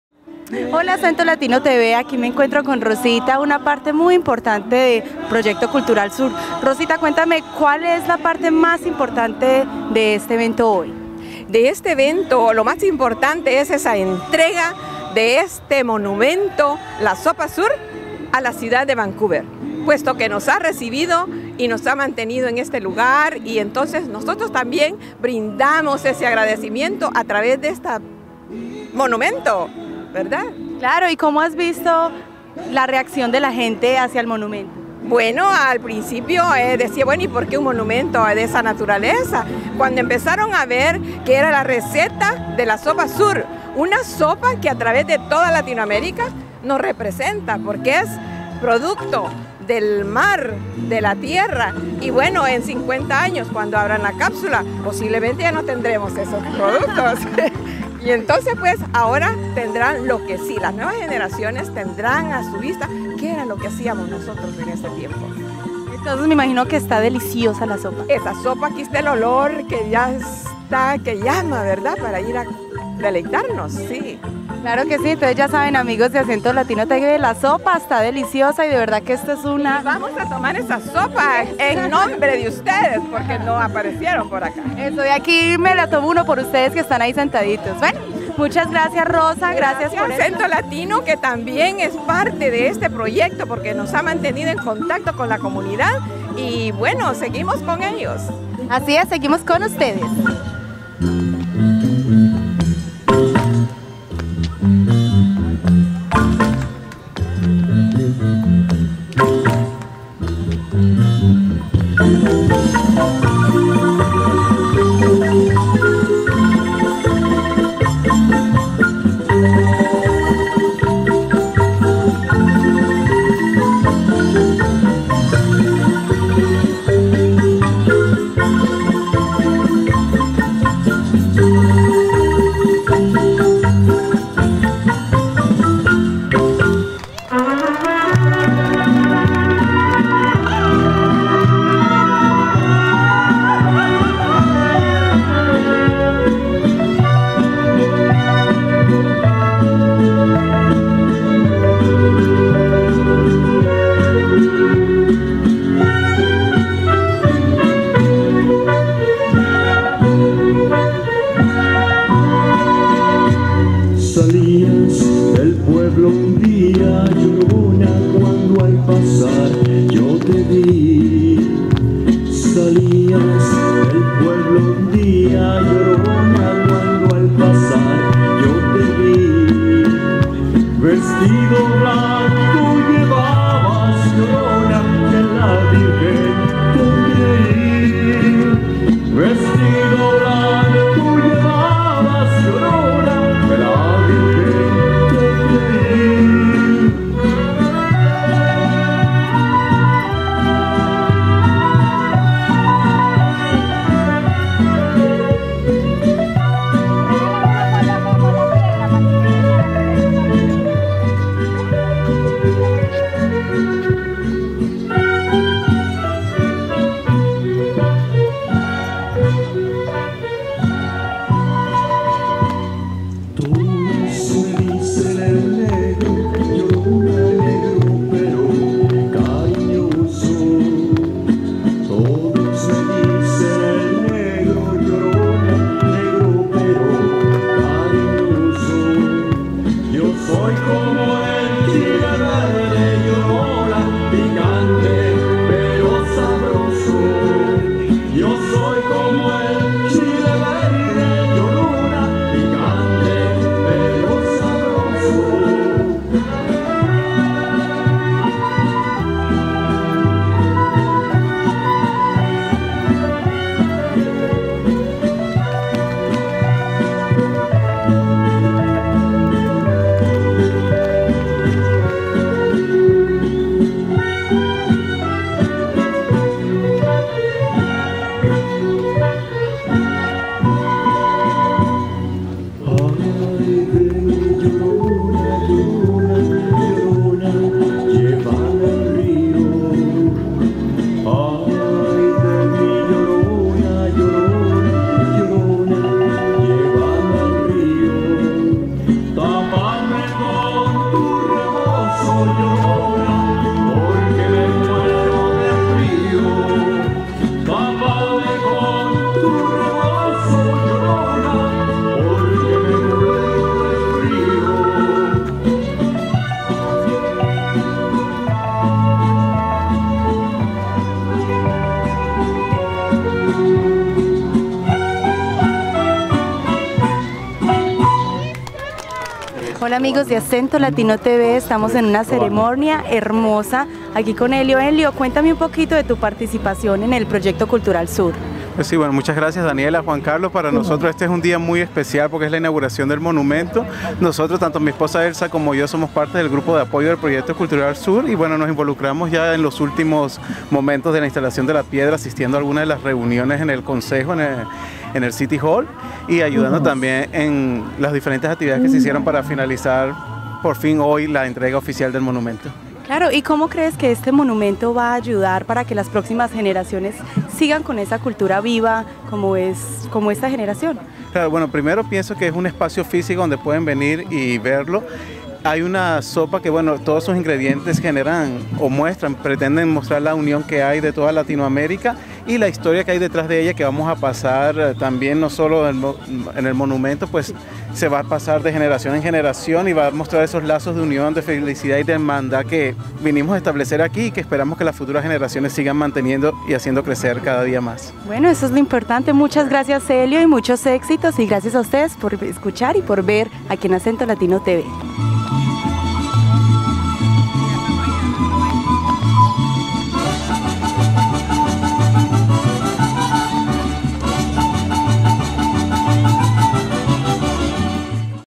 Monumento Sopa Sur - Rosa Brand Canadas + Mexico Vivo + Elio Masci
July 28 2013 Seaforth Peace Park Vancouver - entrega del Monumento Sopa Sur a la ciudad de Vancouver, a gift from the Latinoamerican Community to the City of Vancouver